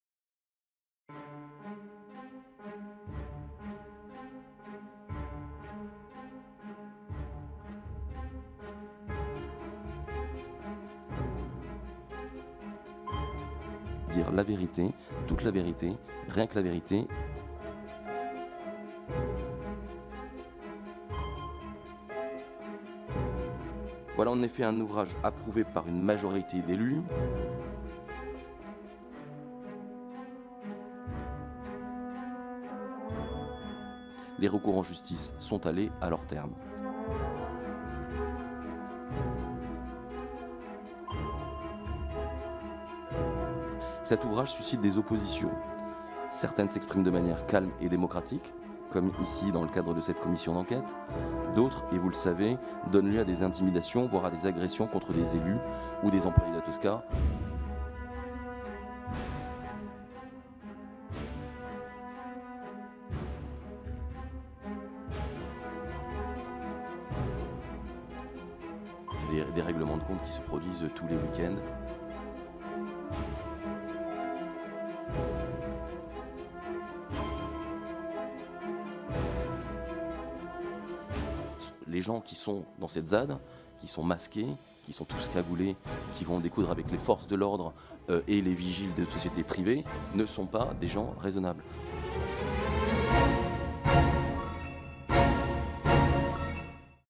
A69 - JURÉ CRACHÉ M. TERLIER ?
🔎Commission d'enquête à l'Assemblée Nationale 🏛️ :
nous nous alarmions de la proximité du président Jean Terlier avec le groupe Pierre Fabre ... A présent nous sommes indignés par ses paroles diffamantes à l'égard des opposants à l'A69 ! Un fact checking s'impose...